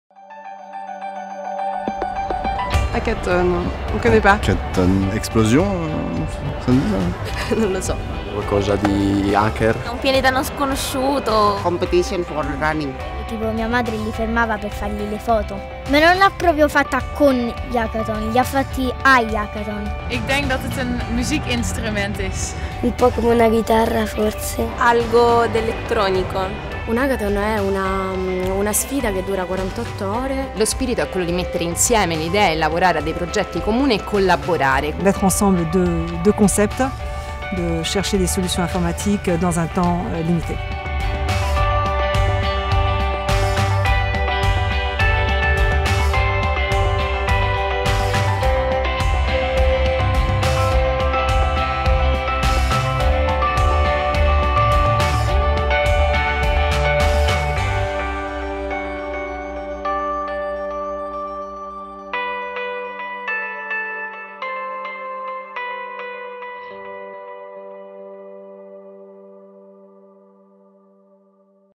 Cosa è un Hackathon? Scoprilo alla Space Apps Challenge!
Cos'è un ‪#‎Hackathon‬? Un'esplosione? un pianeta sconosciuto? una chitarra?

... Per fortuna i nostri appassionati sanno benissimo cos'è: l'11 e 12 aprile torna l'Hackathon ‪#‎SpaceApps‬ Challenge. In Italia puoi partecipare a Roma e a Napoli. Iscritviti qui